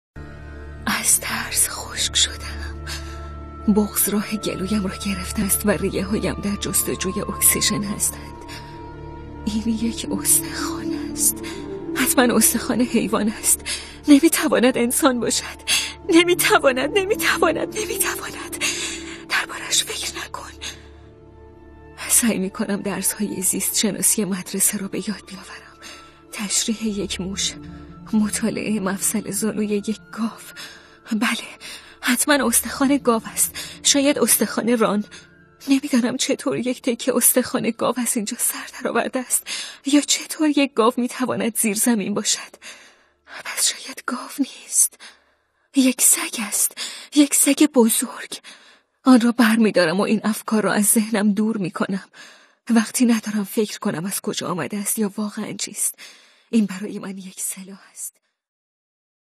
کتاب صوتی پشت سرت را نگاه کن
کتاب صوتی پشت سرت را نگاه کن اثر سی بل هاگ ترجمه آرتمیس مسعودی با صدای آنالی طاهریان. با مراجعه به وبسایت یا اپلیکیشن آوانامه از طریق لینک زیر نسخه کامل این کتاب صوتی را دانلود کنید